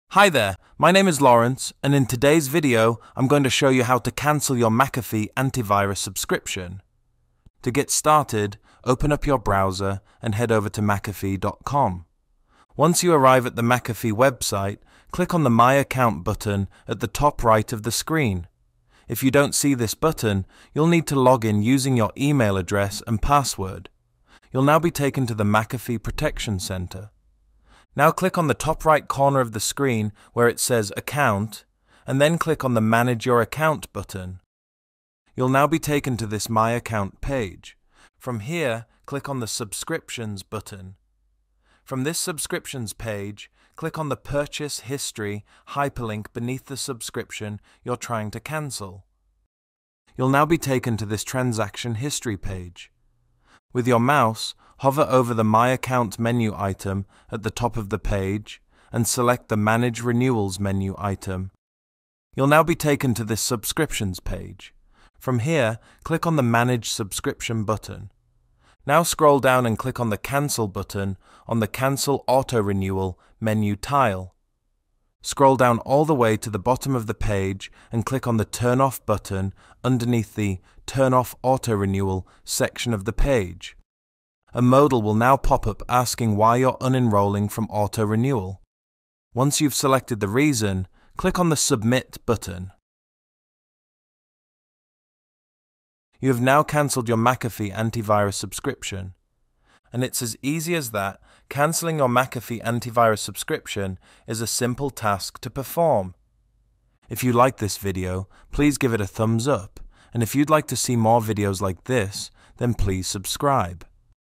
How to Cancel Your McAfee Antivirus Subscription
In today’s video I’m going to show you how to cancel your McAfee antivirus subscription.

1. Open up your browser and head over to McAfee.com.
2. Once you arrive at the McAfee website, click on the My Account button at the top right of the screen.
3. You’ll now be taken to the McAfee protection center.
4. Now click on the top right corner of the screen where it says “Account”, and then click on the “Manage your account” button.
5. You’ll now be taken to this “My account” page.
6. From here click on the “Subscriptions” button.
7. From this subscriptions page, click on the “Purchase History” hyperlink beneath the subscription you’re trying to cancel.
8. You’ll now be taken to this transaction history page.
9. With your mouse, hover over the “My Account” menu item at the top of the page, and select the “Manage Renewals” menu item.
10. You’ll now be taken to this subscriptions page.
11. From here, click on the “Manage subscription” button.
12. Now scroll down and click on the “Cancel” button on the “Cancel auto-renewal” menu tile.
13. Scroll down all the way to the bottom of the page and click on the “Turn Off” button underneath the “Turn off auto-renewal” section of the page.
14. A modal will now popup asking why you’re unenrolling from auto-renewal. Once you’ve selected the reason, click on the “Submit” button.
15. You have now canceled your McAfee antivirus subscription.

And it’s as easy as that, canceling your McAfee antivirus subscription is a simple task to perform.